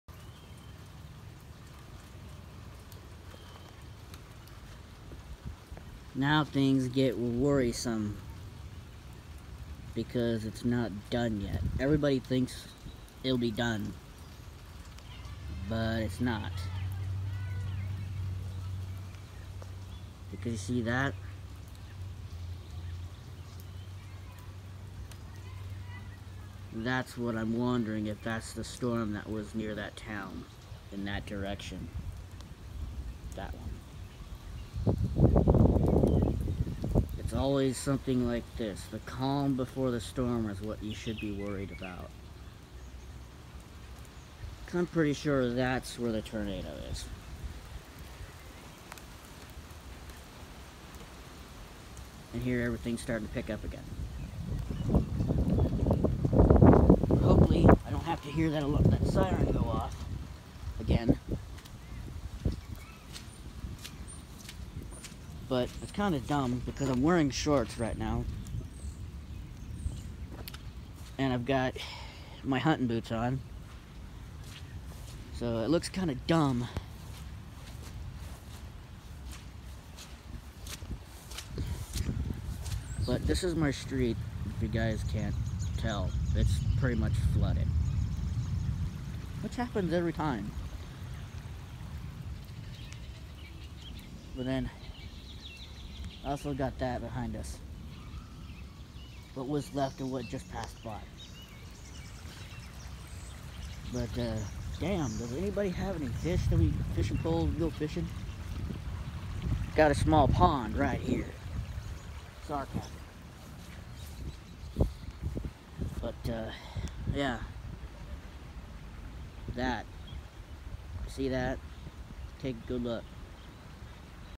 Mother Nature is pissed
Flooded street was in a tornado watch. Tornado did pass by rained enough to flood my street